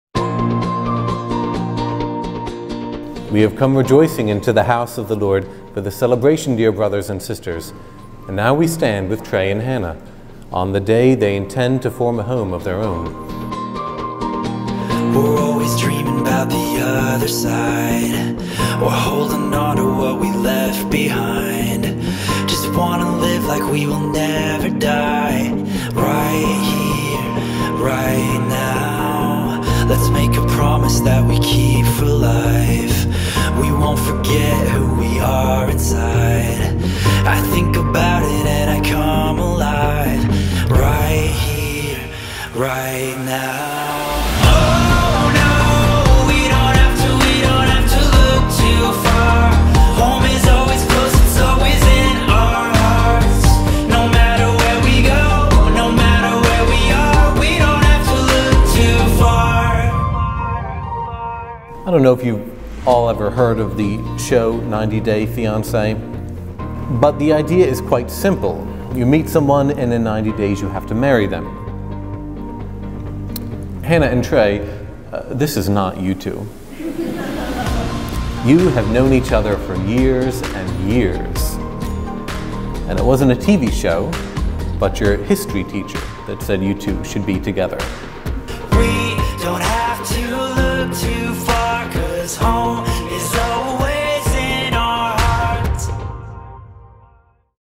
"It wasn't a TV show but your History teacher that said you two should be together" (A OCMD Wedding)
A sneak peek at Hannah & Trey's beautiful wedding in Ocean City, Maryland! ❤️